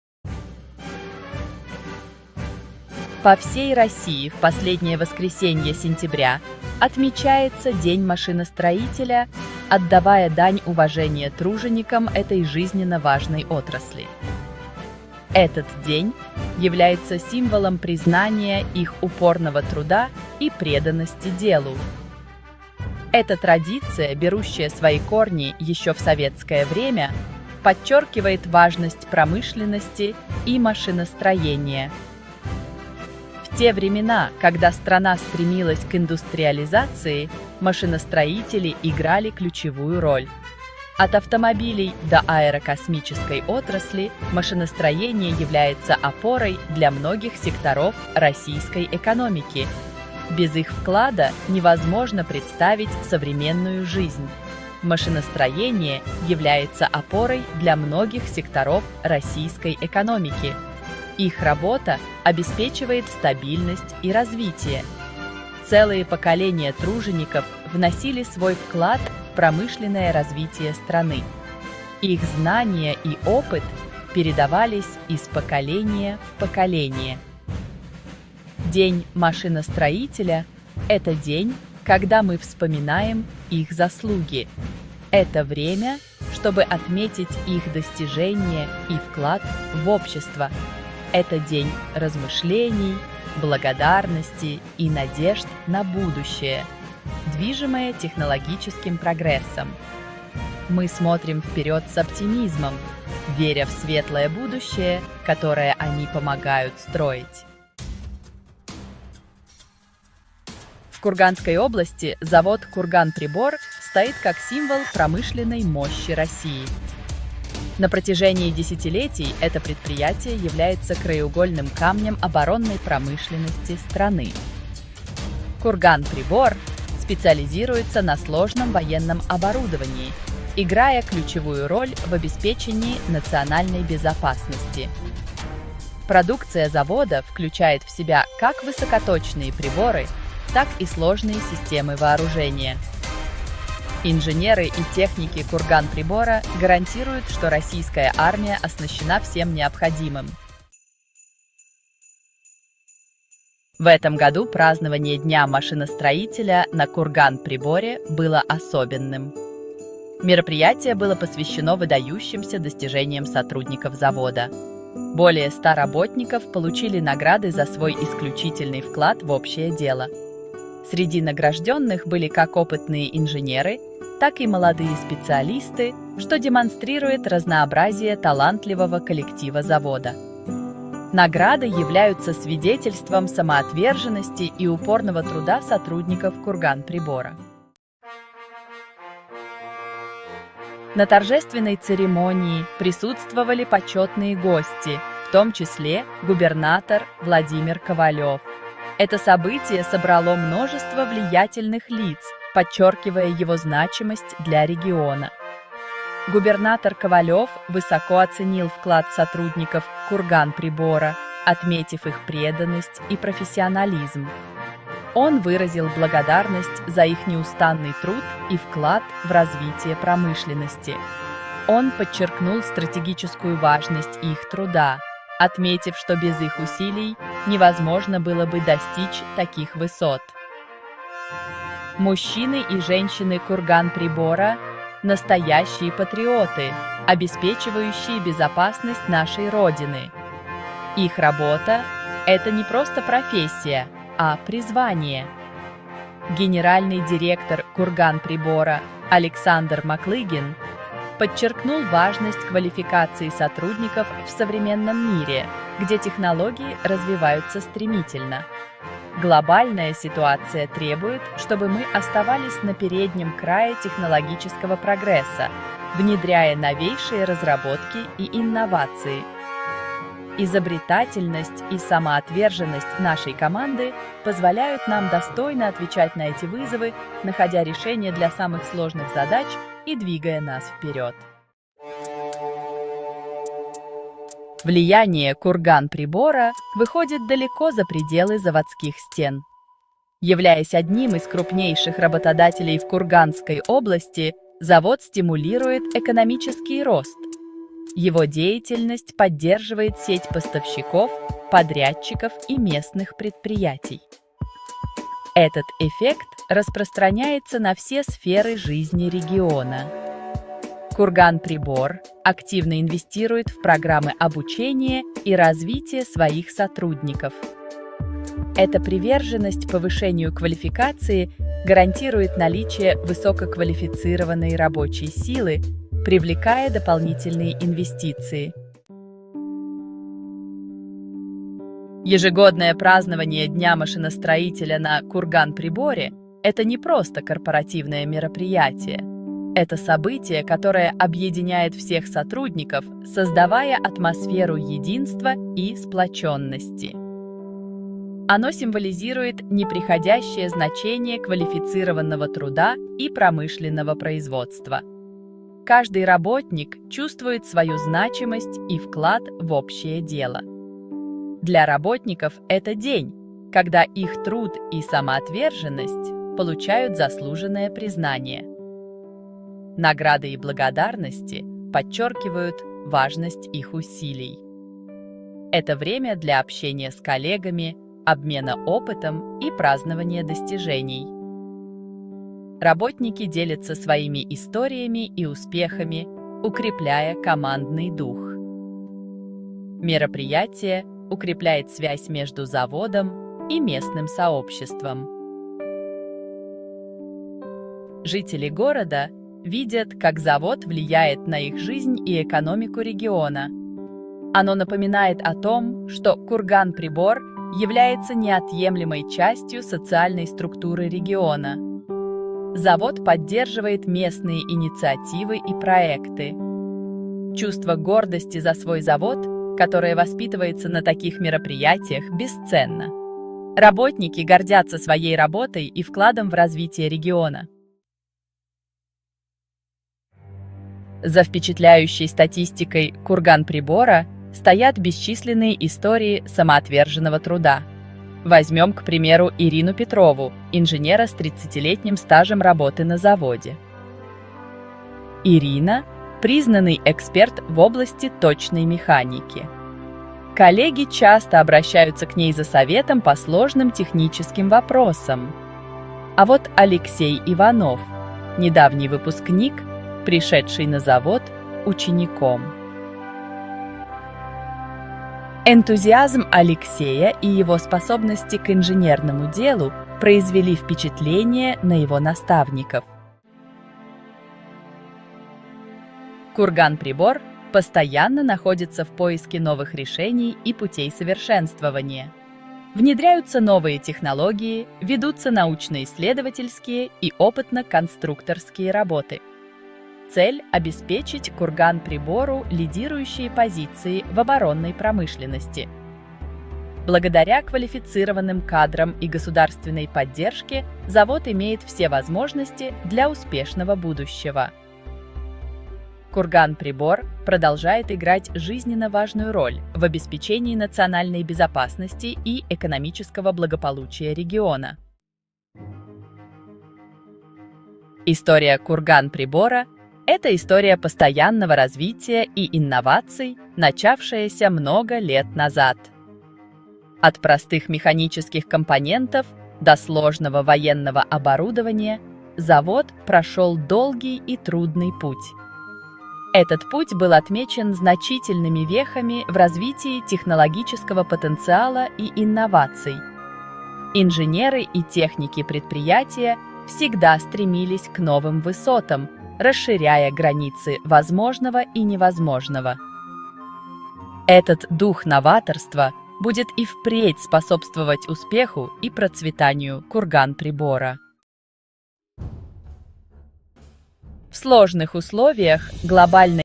Поздравление сотрудников «Курганприбора» с Днем
OUTLINE:
00:00:00 Celebrating Machine Builders in Russia
00:01:47 A Bastion of Defense Production
00:02:30 Awards and Recognition
00:03:07 Praising Dedication and Skill
00:04:41 Kurganpribor's Impact on the Region
00:05:28 Boosting Morale and Regional Pride
00:07:07 Stories of Dedication
00:07:53 Kurganpribor's Future Endeavors
00:08:34 From Past to Present
00:09:21 Kurganpribor's Role in Russia's Industrial Landscape

Поздравление сотрудников «Курганприбора» с Днем машиностроителя! 🎉

**Description (Russian):**
🎊 В этом видео мы отмечаем важное событие — День машиностроителя! 🎉 На предприятии «Курганприбор» состоялось торжественное награждение сотрудников, которые внесли значительный вклад в развитие отрасли. Мы расскажем о высоких наградах и благодарностях, которые были вручены 86 заводчанам. 🏆

📅 В этом году праздник пройдет 29 сентября, и мы рады поделиться с вами атмосферой этого важного дня! 🌟 Узнайте больше о достижениях «Курганприбора» и его роли в экономике Курганской области. 💼

**Description (English):**
🎊 In this video, we celebrate an important event — the Day of Mechanical Engineer! 🎉 A solemn award ceremony took place at the "Kurganpribor" enterprise where employees who made a significant contribution to the industry were honored. We will share the high awards and acknowledgments presented to 86 factory workers. 🏆

📅 This year, the holiday will be held on September 29, and we are excited to share with you the atmosphere of this significant day! 🌟 Learn more about the achievements of "Kurganpribor" and its role in the economy of the Kurgan region. 💼